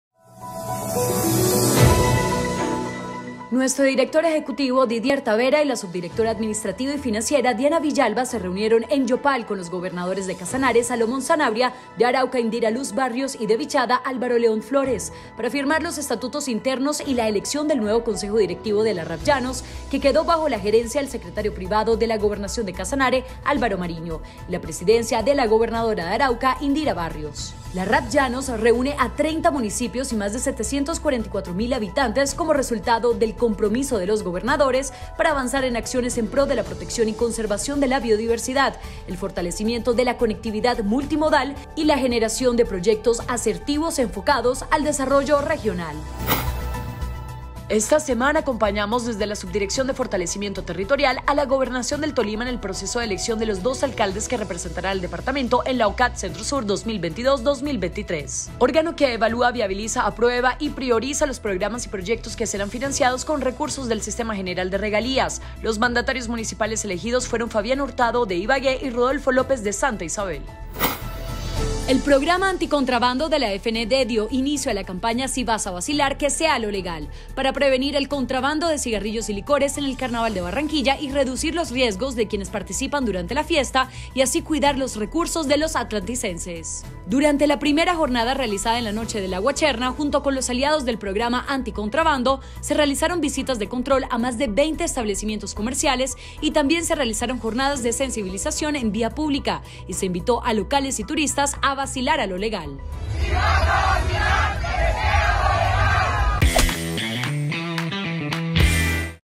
Las BrevesDeLaSemana | del 14 al 18 de marzo
🔹RAPLlanos establece su estructura administrativa

🔹Tolima eligió representantes en el OCAD Centro-Sur

🔹En Carnaval2022 si vas a vacilar ¡que sea sea a lo legal!

Noticias en 🎥